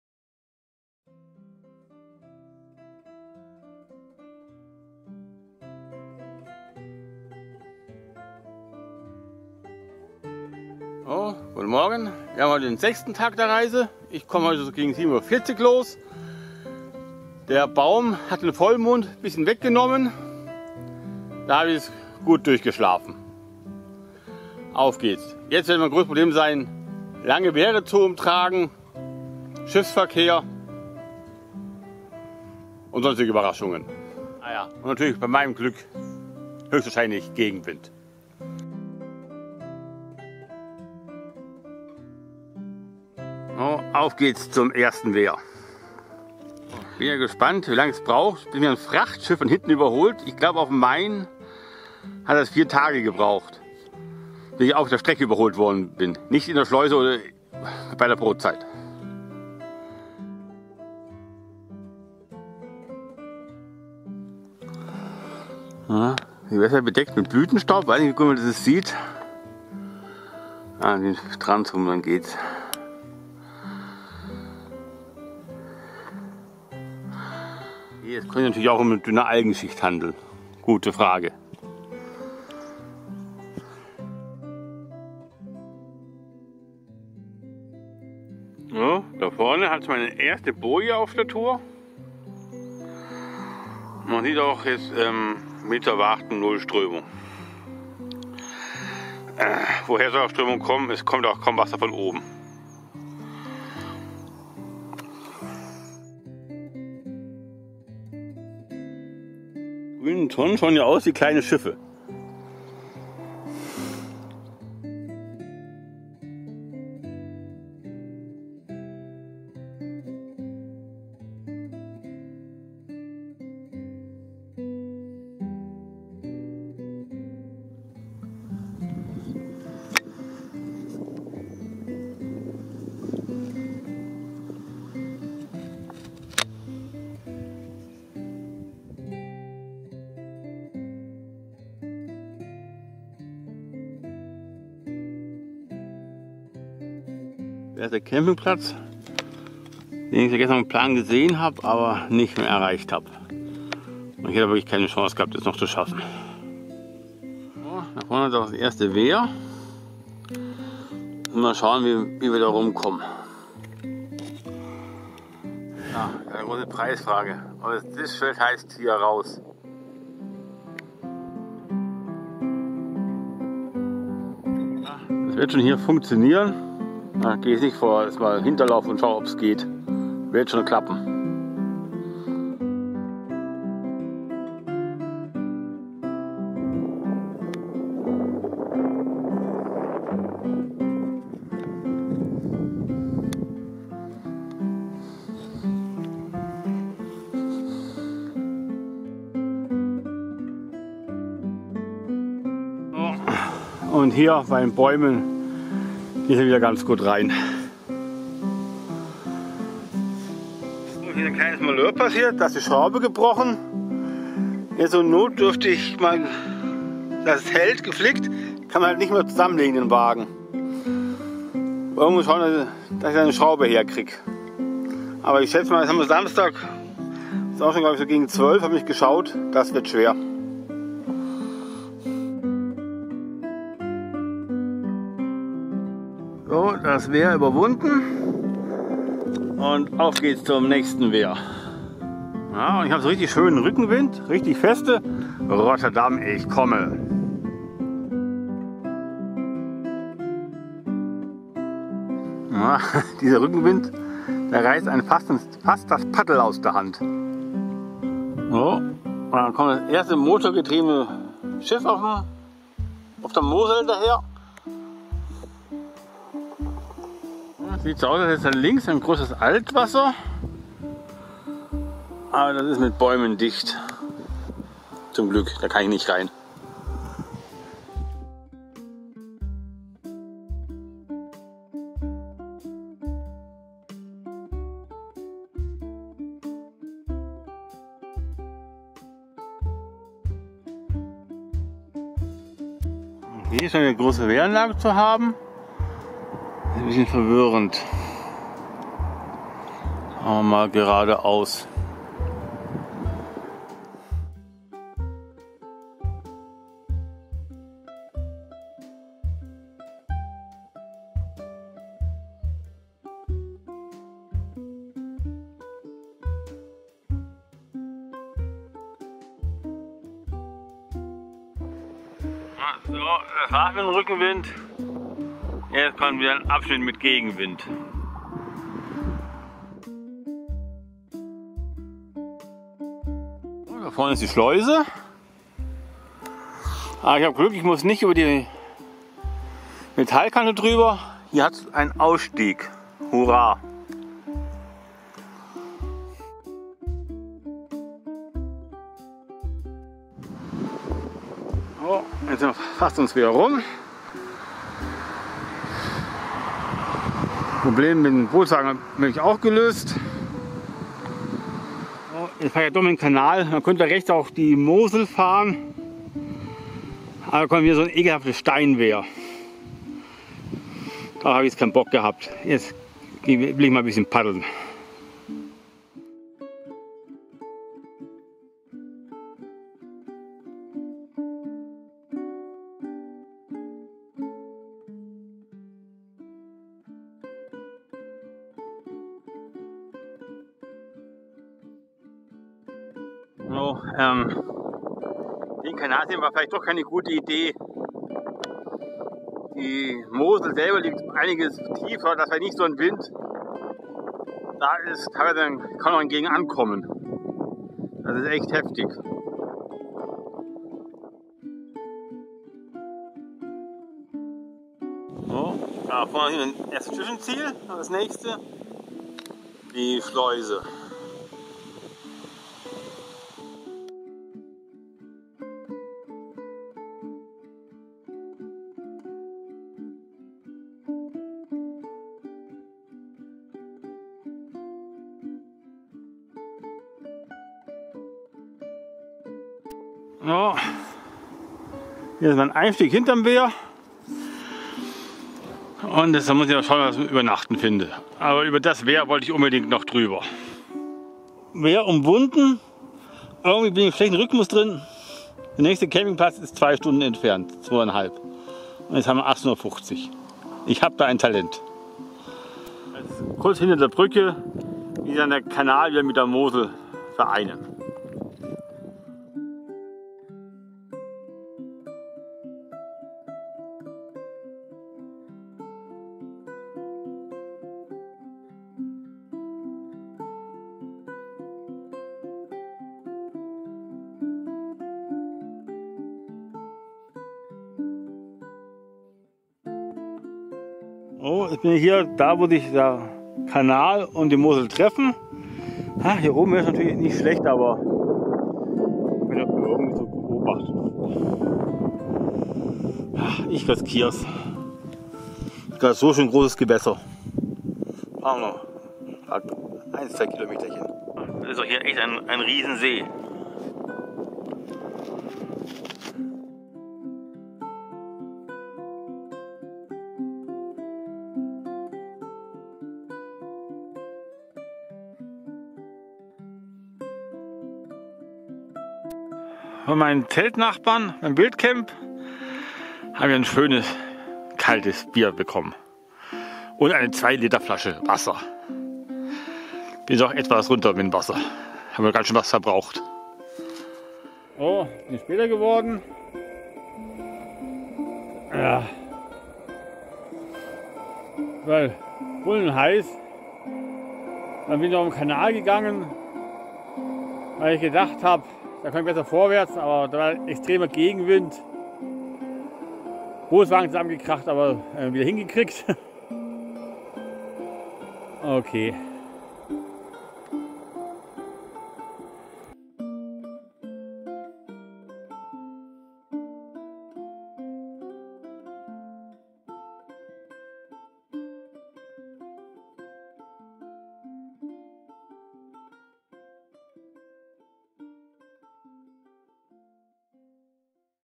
6/18 3 Länder Kajaktour auf der Mosel. (Moselle) Von Épinal Frankreich nach Koblenz Deutschland.
Film Documentation über den 6.Tag der Kajak Tour auf dem Fluss Mosel (Moselle) in Frankreich.
Der Start erfolgte um 07:40 Uhr kurz vor der französischen Ortschaft Maron aus. Wo ich auf einer Wiese neben den Fluss die Nacht in meinem Zelt verbracht habe. Die Reise mit dem Schlauchkajak Seawave von Gumotex führte mich Flusswanderer bis in die Nähe von Aingeray. Wo ich auch wieder in der Wildnisse Biwakierte habe. Zurückgelegt habe ich Wasserwanderer an diesem Expeditionstag mit dem Luftkajak auf den französischen Fluss der Moselle etwa 27,5 Km.
Auf der Kanalmosel war mit einem dichter Algen Teppich der das Tourenkajak gut Bremste. Wenn die Algen nicht durch Schiffsverkehr aufgerissen wurden bildeten sie eine feste Schicht auf dem Wasser. Die 1. Schleuse die mit dem Reisekajak Umgangen werden musste an diesem Tag der Flusswanderung war beim Wehr Villey-le-Sec. Das Ich auf der linken Seite Umgangen habe. Das nächste Hindernis war die Schleuse bei Toul. Wo es dank einem Boot Verleih ein gutes Aus / Einstiegstelle hat. Die sich Allerdings auf verschiedenen Seiten vom Fluss befinden. Hinter der Wehr bin ich dann auf den Mosel Kanal gefahren. Am Ende vom Kanal musste noch das Wehr  Fontency auf der linken Seite Umgangen werden. Kurz nach dem sich Mosel und Kanal wieder vereinigen bilden sie einen See. Dort habe Ich in der Nähe von Aingeray auf einer Wiese im Zelt übernachtet. An diesem 6 Tag der Kayak Expedition wechselte Starker Rückenwind und Gegenwind ständig ab.
___________Kapital___________
00:00     Einleitung
02:40     Schleuse   Villey-le-Sec.
05:36     Schleuse   Toul.
06:57     Schleuse   Fontency-sur-Moselle
08:51     Ausleitung